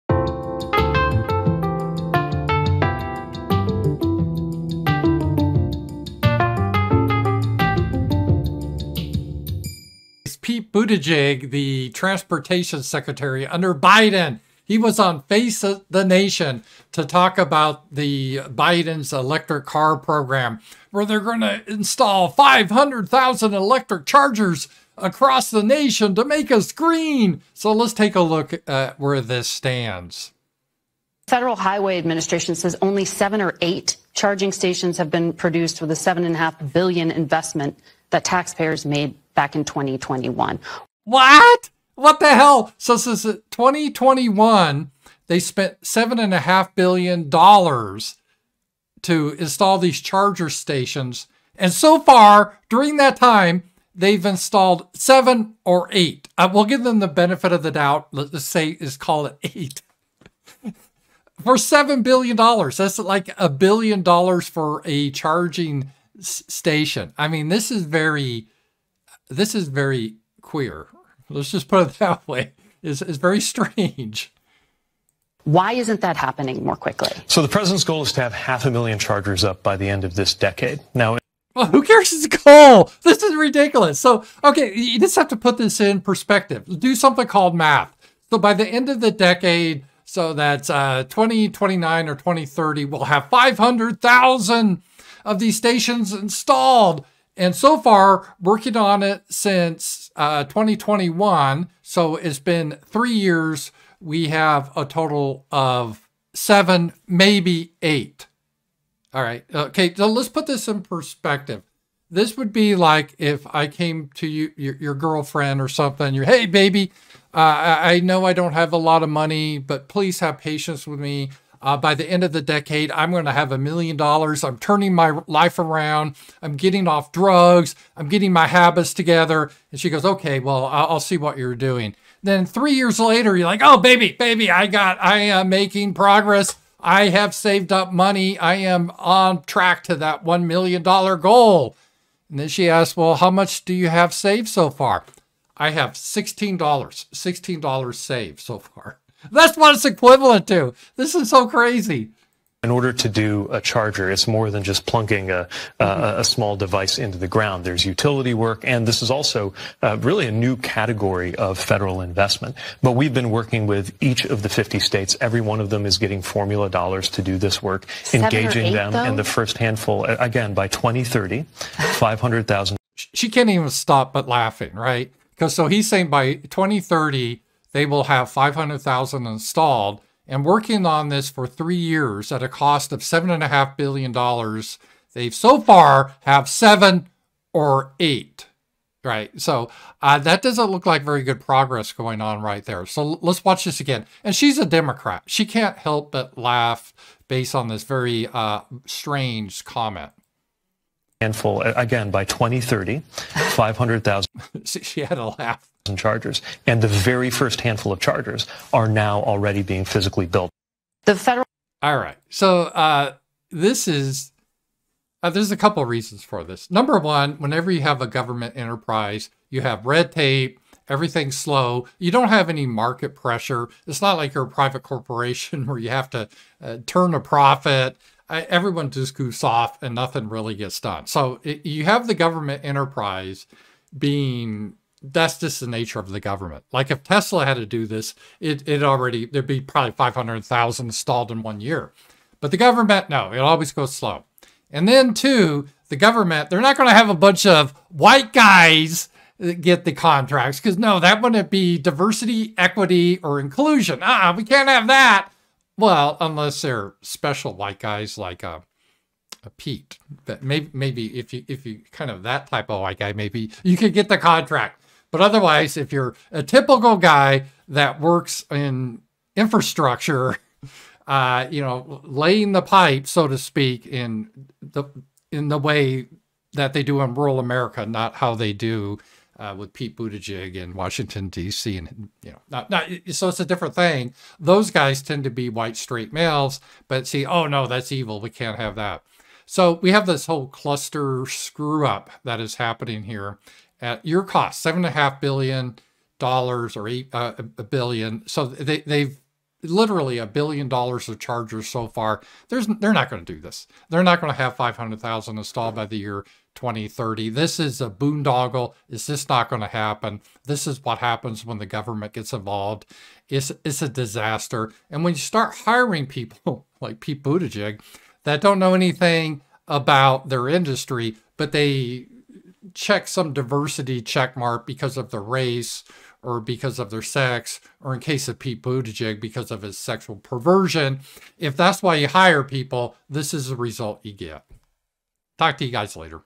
EV Disaster!
Only 7 EV charging stations produced in 3 years instead of 50,000 per year to meet goal: Transportation Secretary Pete Buttigieg on CBS Struggles To Explain Slow Rollout of EV Charging Stations Despite $7.5 Billion Investment: 'It's More Than Just Plunging a Device into the Ground' — Only 7 Produced So Far, Need 50,000 Per Year to Meet Goal.

Join Gumroad  or GroupApp to participate in our weekly private roundtables and other perks!